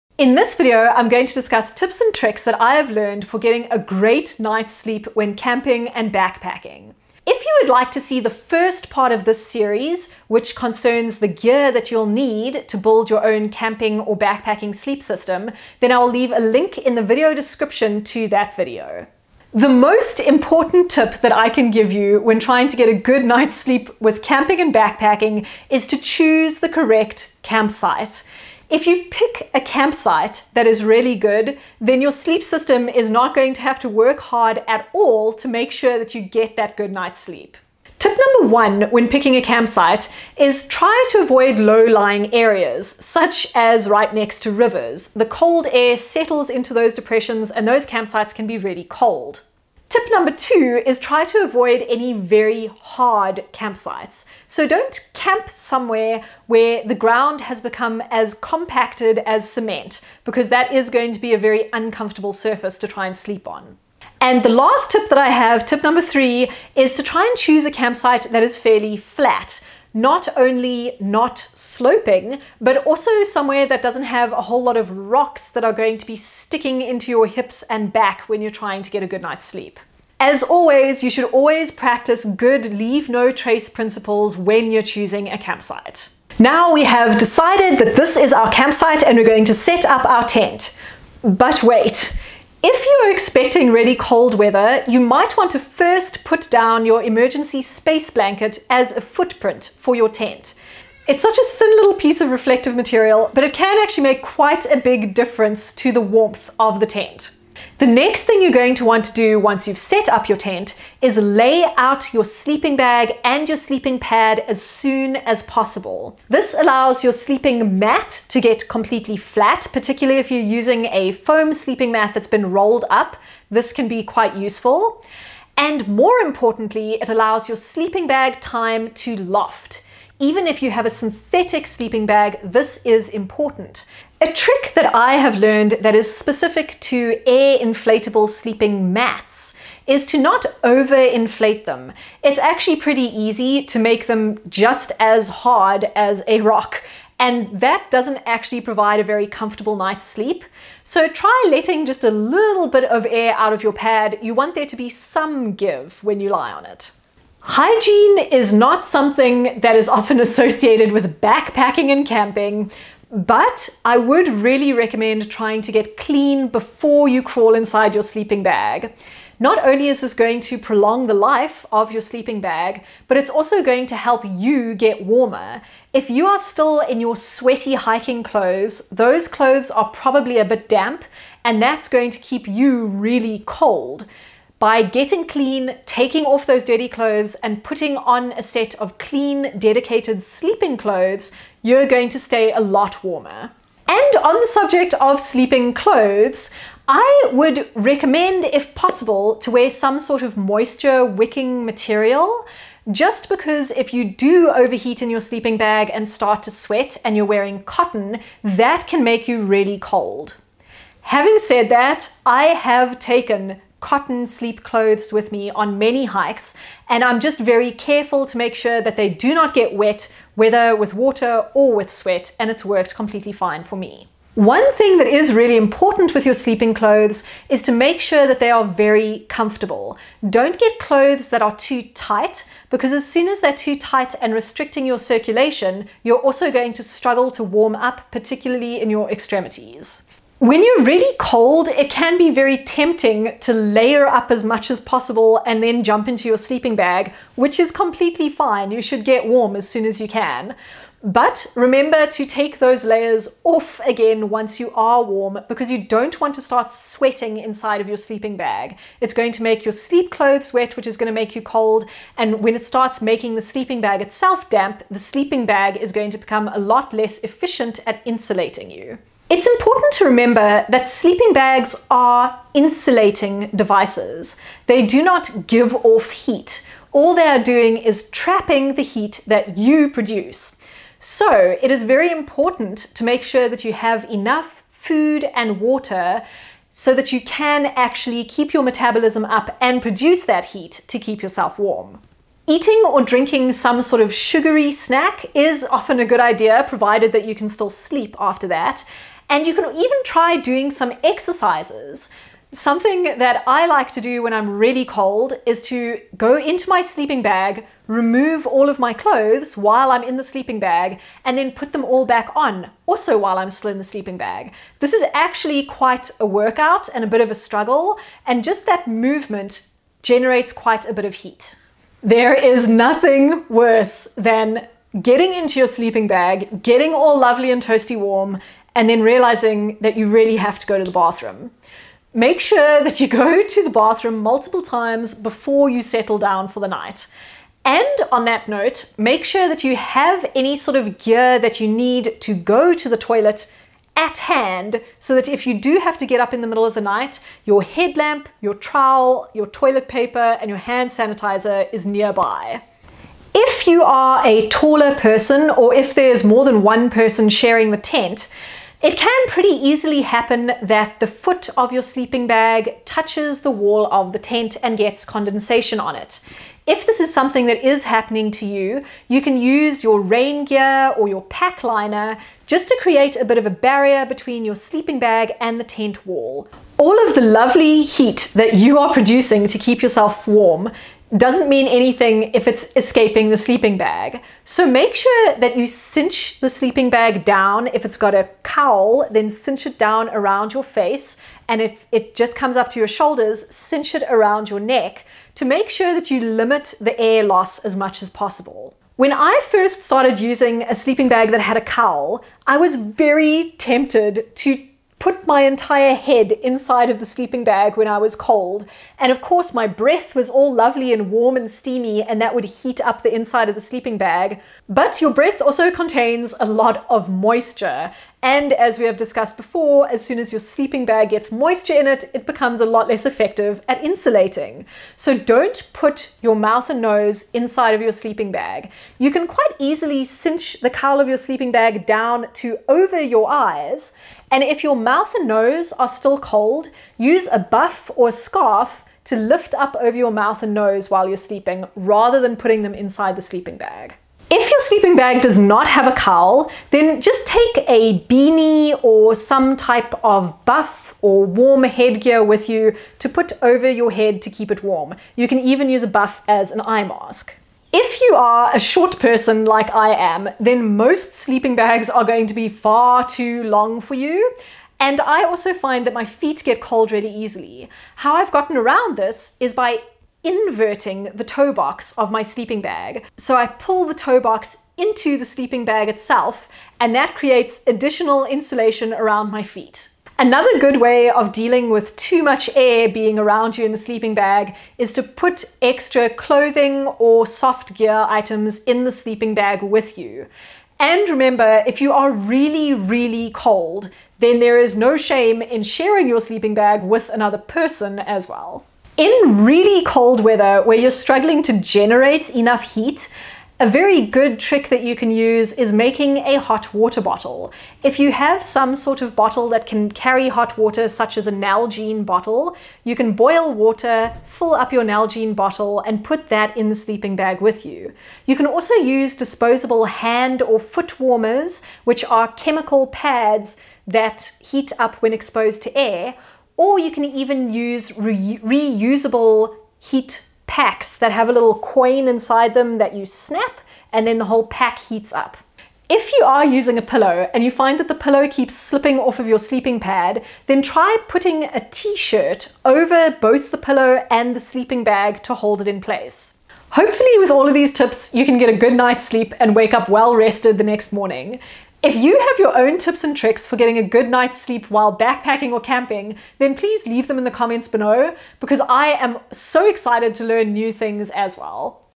Backpacking Sleep Tips and Tricks
Sleeping in a tent during a backpacking or camping trip can be uncomfortable and cold, but not if you try out these tips and tricks! PART ONE of this series (about sleep gear) can be found